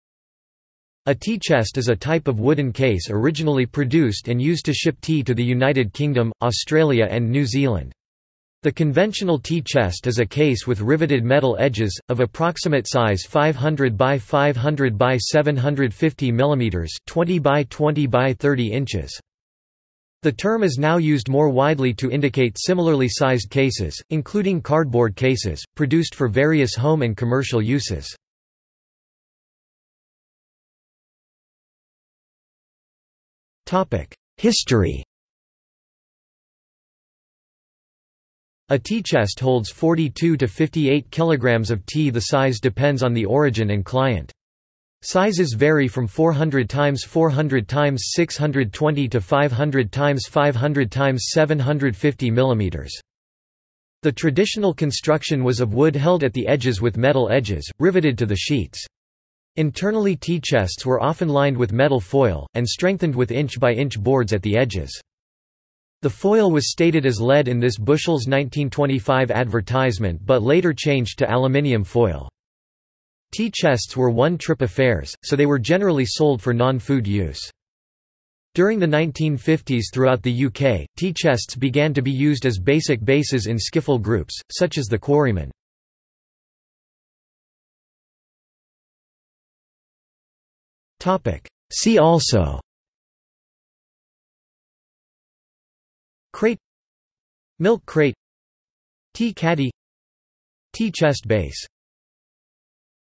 Tea chest | Wikipedia audio article
This is an audio version of the Wikipedia Article:
Tea chest

00:00:30 undefined
00:01:27 undefined

- Socrates

SUMMARY
A tea chest is a type of wooden case originally produced and used to ship tea to the United Kingdom, Australia and New Zealand. The conventional tea chest is a case with riveted metal edges, of approximate size 500x500x750 millimetres (20x20x30 inches).
The term is now used more widely to indicate similarly-sized cases, including cardboard cases, produced for various home and commercial uses.